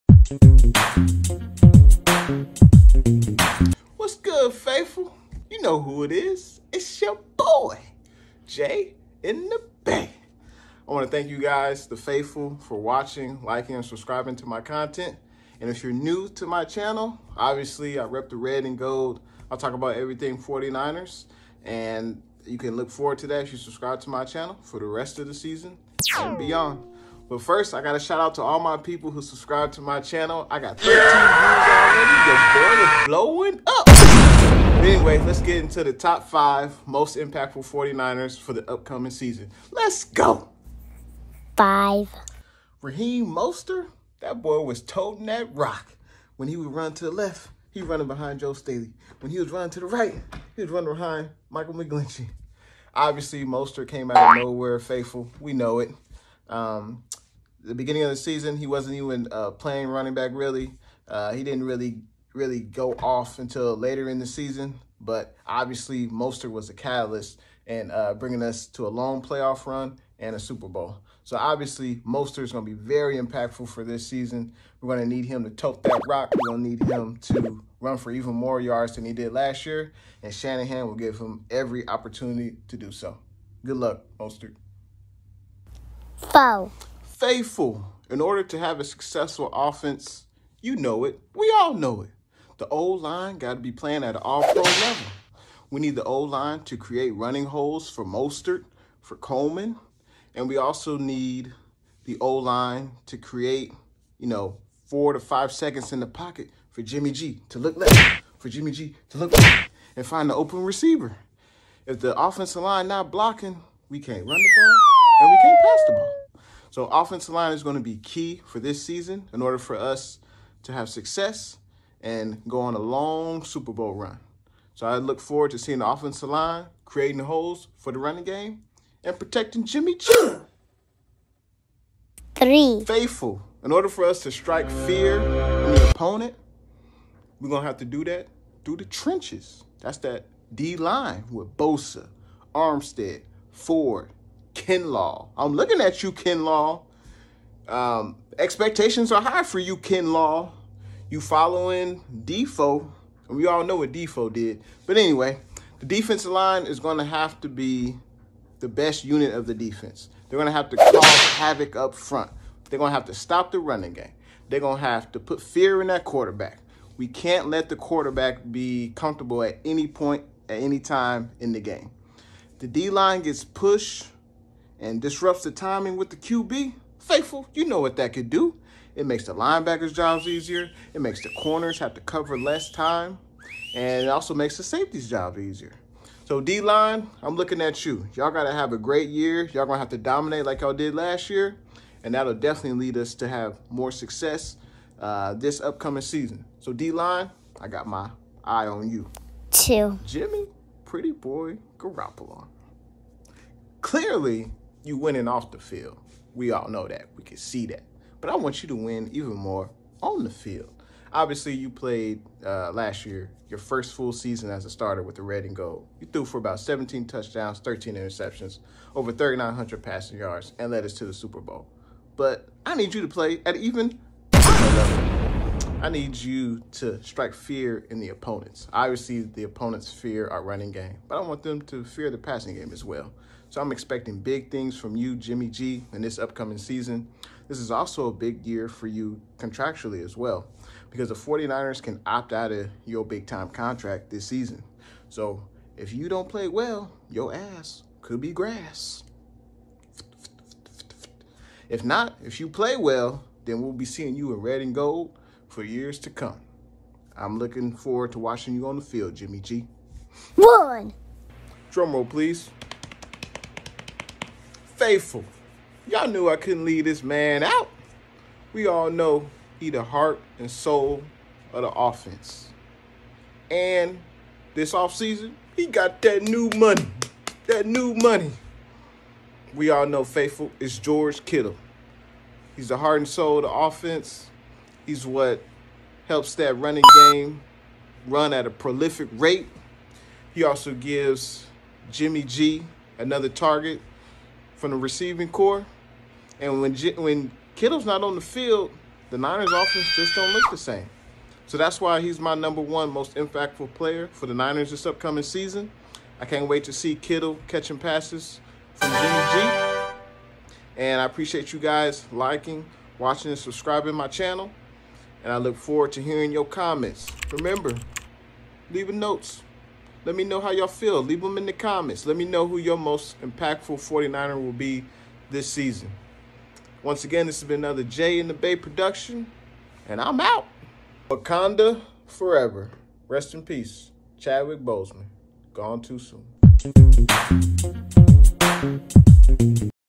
Top 5 Impactful 49er Players 2020 Season
Greetings faithful! Thank you for tuning in to my list of top 5 impactful 49er players for the upcoming season.